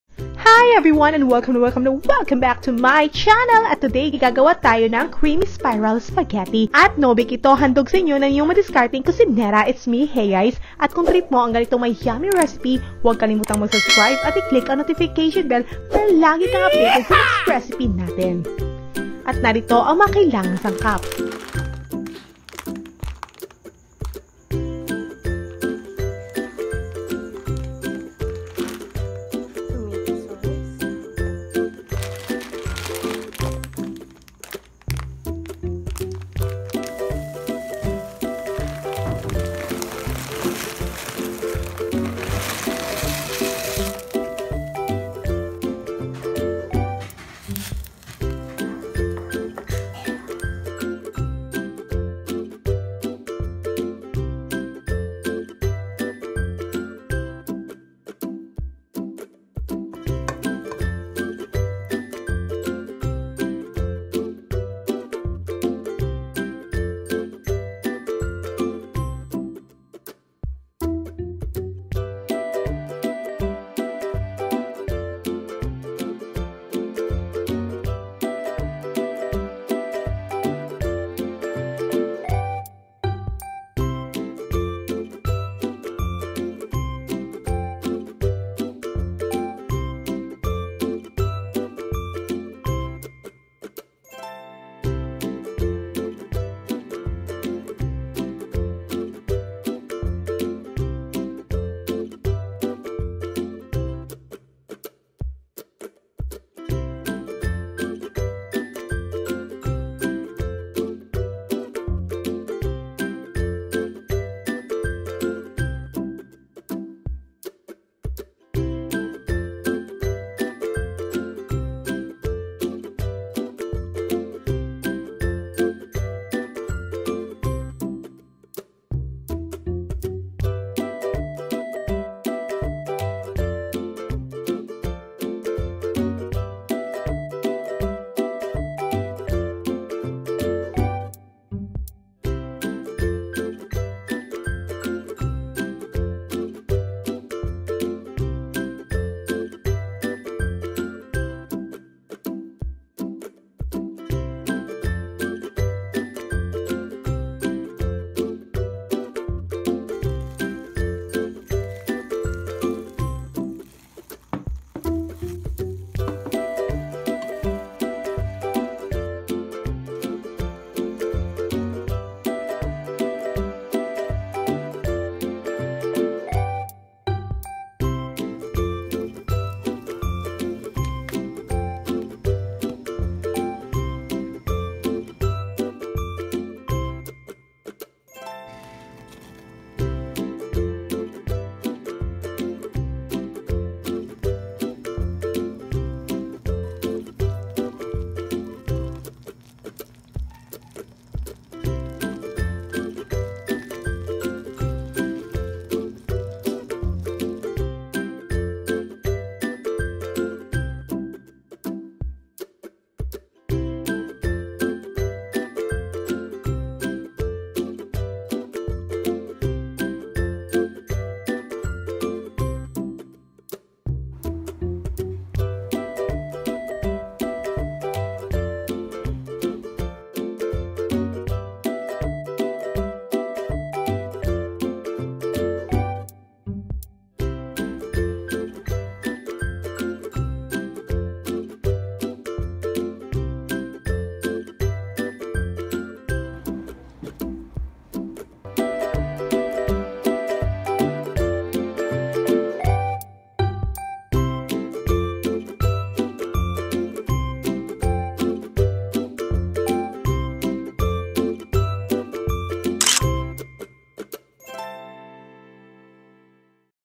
How to cook baked spag without Oven |SPIRAL PASTA NO BAKE by Heyaiz
Hi guys today bigyan natin ng twist ang nakaugaliang spaghetti!
Try natin ang spiral pasta at gawing Creamy and Cheesy!
No Bake ito!

 0:19: INGREDIENTS